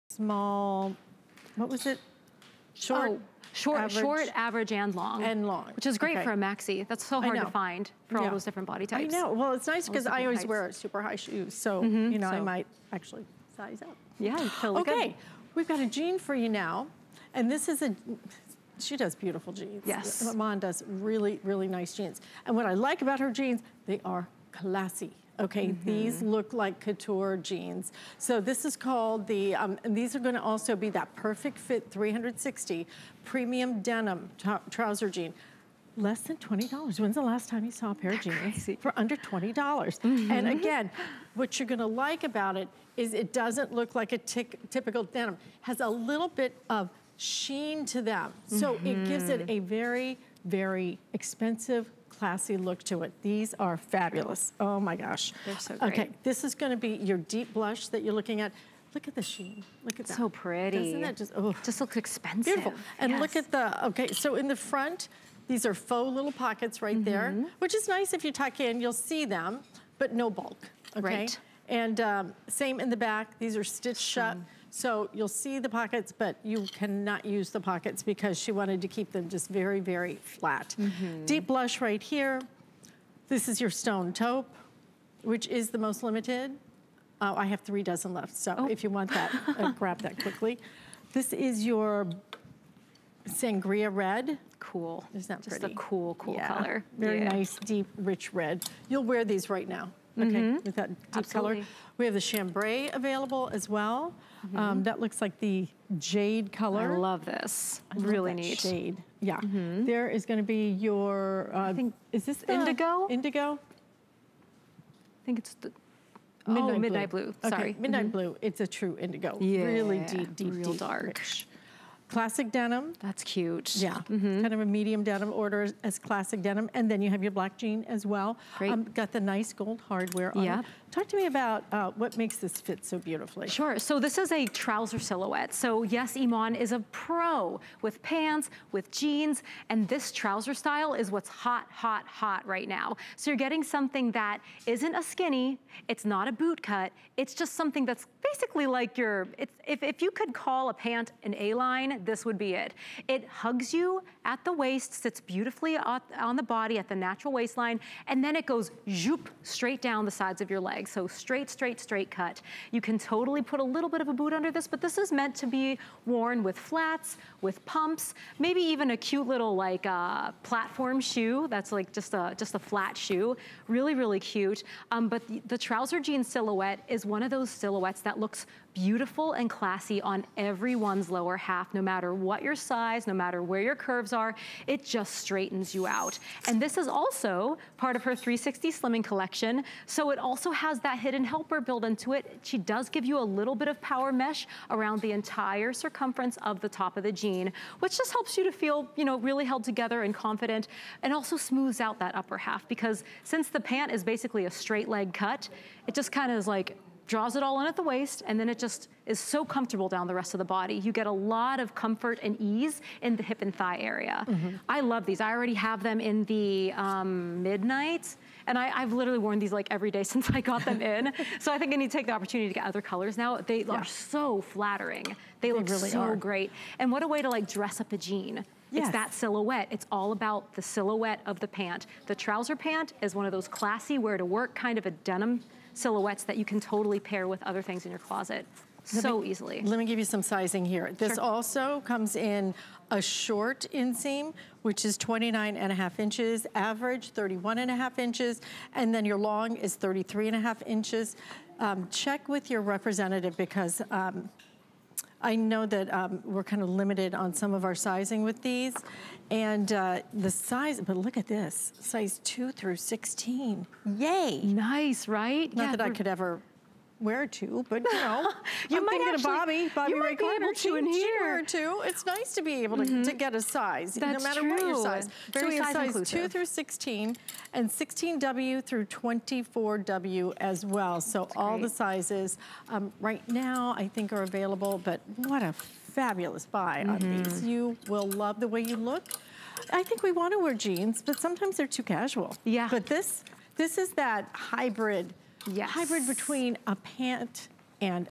IMAN Perfect Fit 360 Premium Denim Trouser Jean
What a great way to smooth things over! Designed with an interior tummy panel, this sophisticated trouser jean flatters your figure in just the...